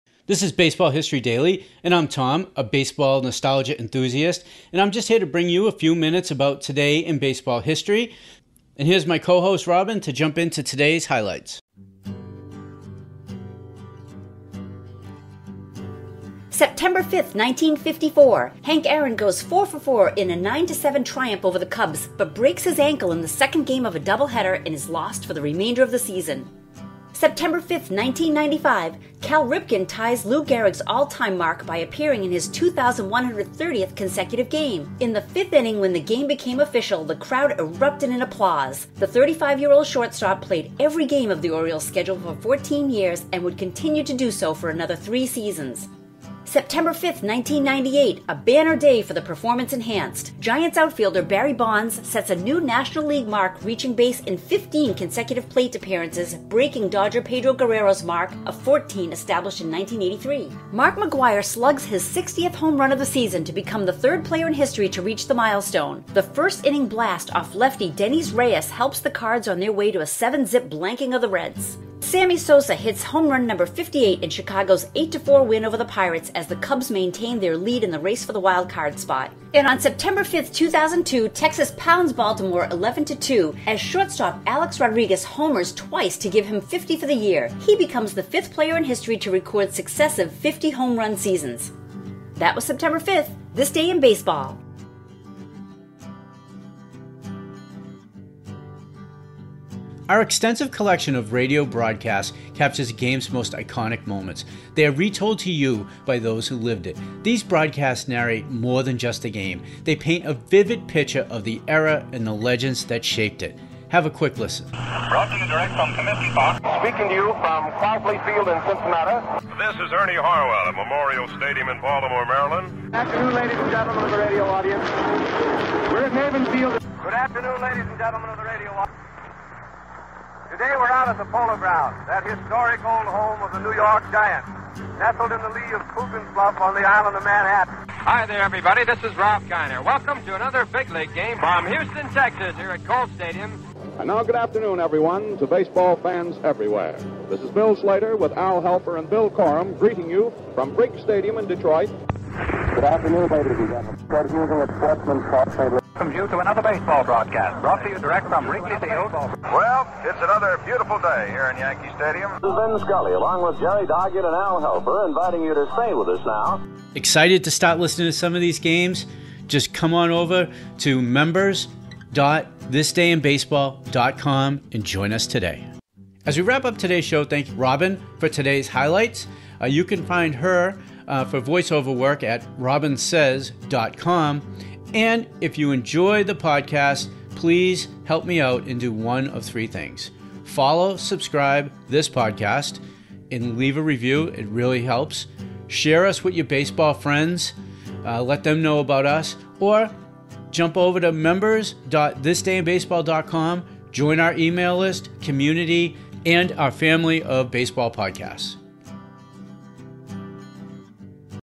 September 5 McGwire slugs his 60th home run
September 5
1954, Hank Aaron goes 4 for 4 in a 9 - 7 triumph over the Cubs, but breaks his ankle in the second game of a doubleheader and is lost for the remainder of the season.

1995, Cal Ripken ties Lou Gehrig's all-time mark by appearing in his 2,130th consecutive game . In the fifth inning, when the game became official the crowd erupted in applause. The 35 year old shortstop played every game of the Orioles schedule for 14 years and would continue to do so for
another three seasons.

1998:  A banner day for outfielder Barry Bonds sets a new National League mark reaching base in 15 consecutive plate appearances breaking Dodger Pedro Guerrero's mark of 14 established in 1983.

McGwire slugs his 60th home run of the season to become the 3rd player in history to reach the milestone. The 1st-inning blast off lefty Dennys Reyes helps the Cards on their way to a 7 - 0 blanking of the Reds.

Sosa hits home run #58 in Chicago's 8 - 4 win over the Pirates, as the Cubs maintain their lead in the race for the wild card spot.

2002 Texas pounds Baltimore 11 – 2 as SS Alex Rodriguez homers twice to give him 50 for the year. He becomes the 5th player in history to record successive 50-home run seasons.